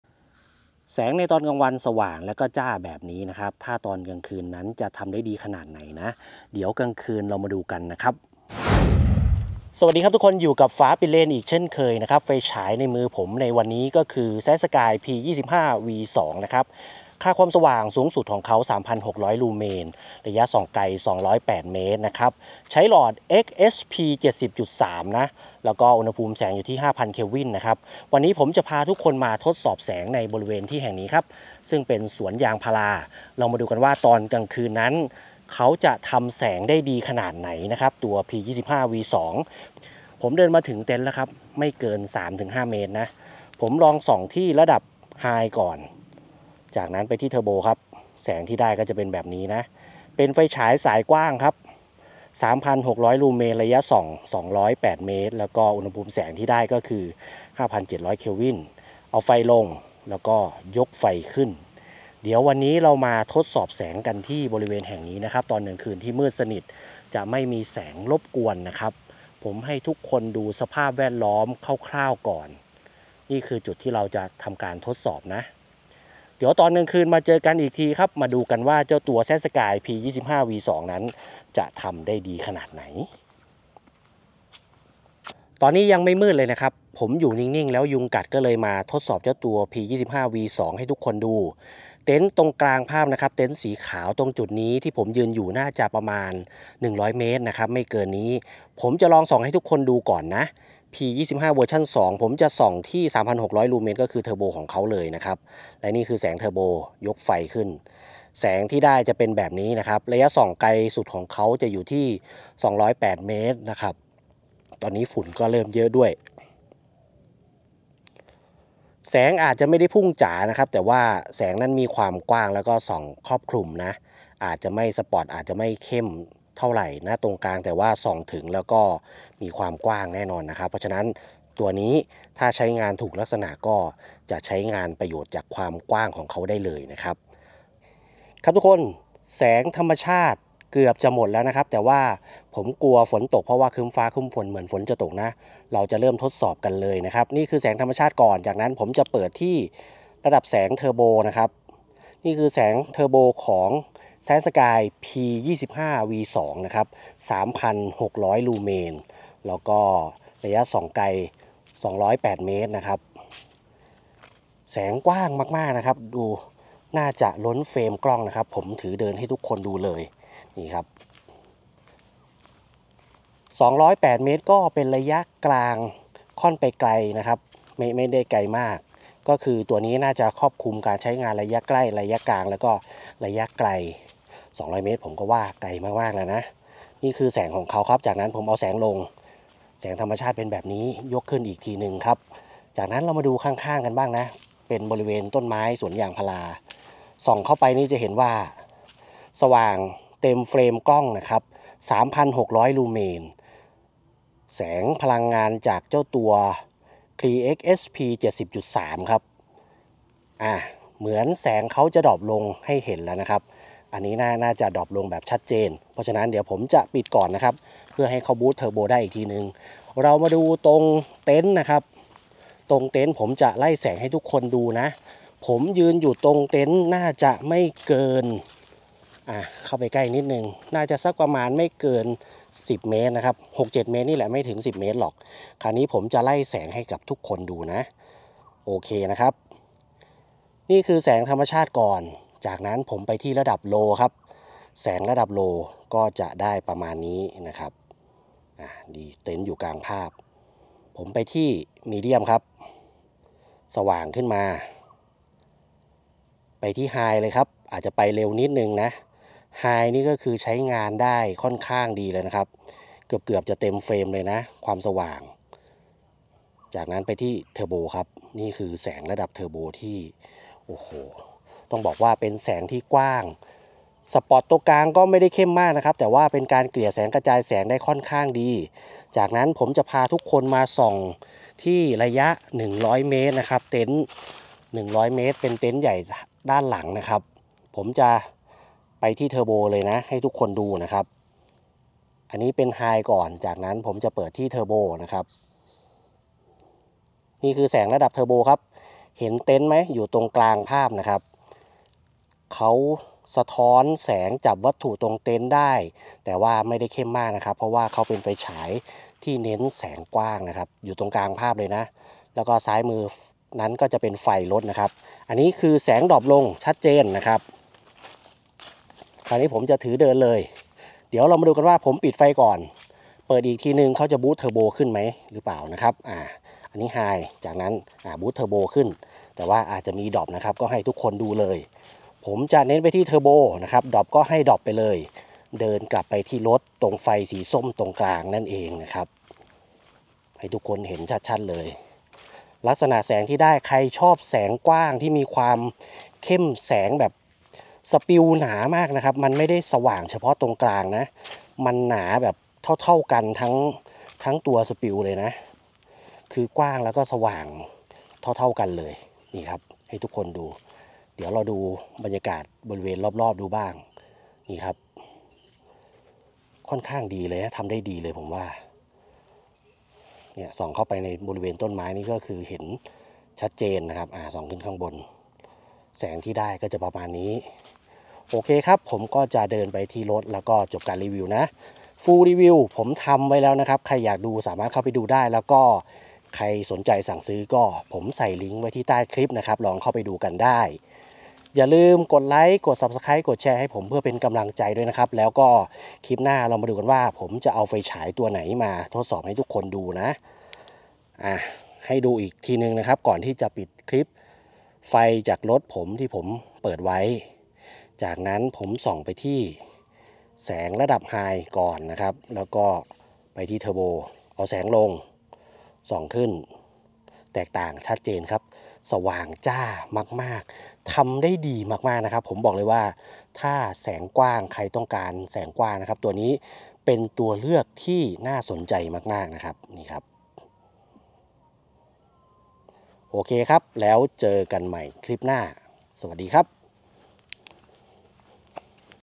รีวิวแสง CYANSKY P25 V2 CREE XHP70.3 แสงกว้างสุดๆ 3600 ลูเมน 208 เมตร ฝาปิดเลนส์รีวิว EP.197
✅ตัวแทนจำหน่ายไฟฉาย Cyansky อย่างเป็นทางการในประเทศไทย สามารถเข้าไปดูตามลิงค์นี้ได้เลยครับ